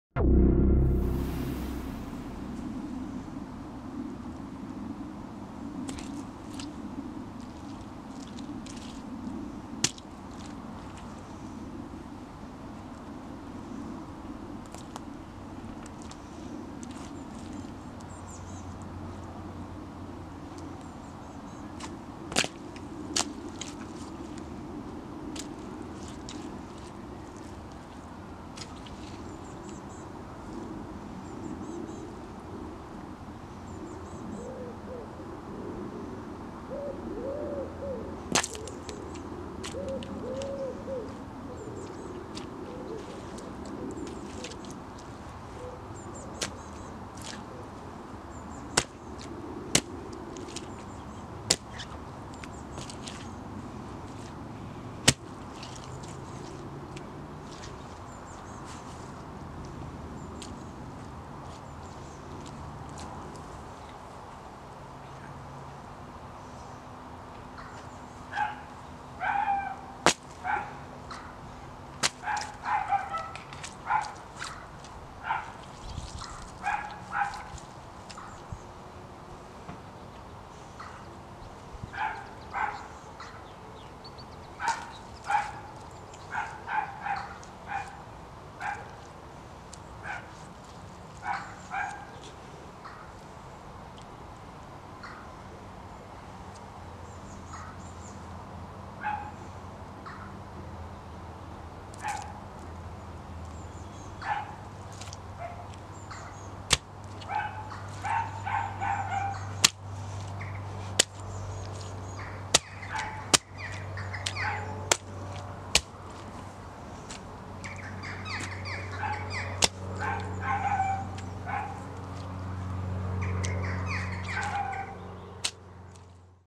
Eva crushes fruit with Doc Martens and Converse
Eva steps on some fruit with her doc martens and changes to converse for a second round!